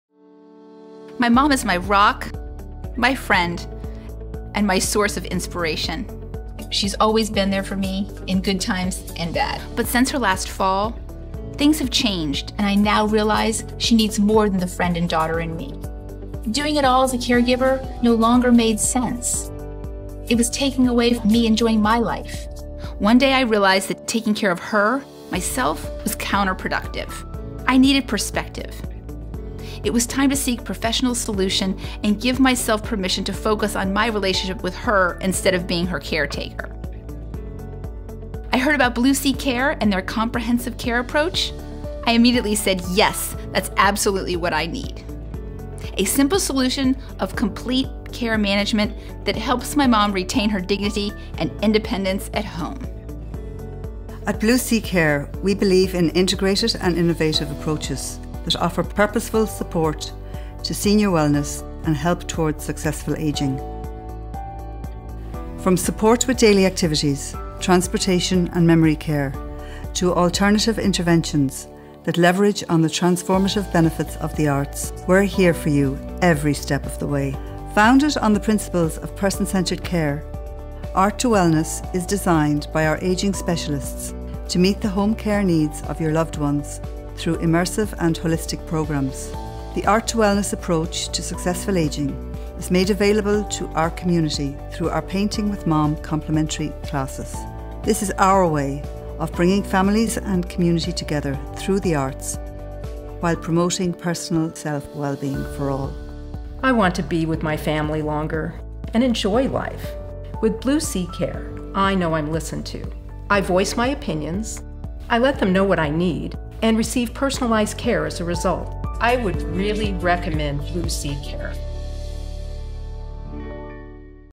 The BlueSea Care Difference: Senior In-Home Care Seal Beach CA
At BlueSea Care, we use innovative processes for person-centered management. Our senior wellness programs use comprehensive care as well as offering assistance with the therapeutic benefits of the arts. Serving Southern California, including Malibu, Seal Beach, Los Angeles, Huntington Beach, Torrance, and surrounding areas.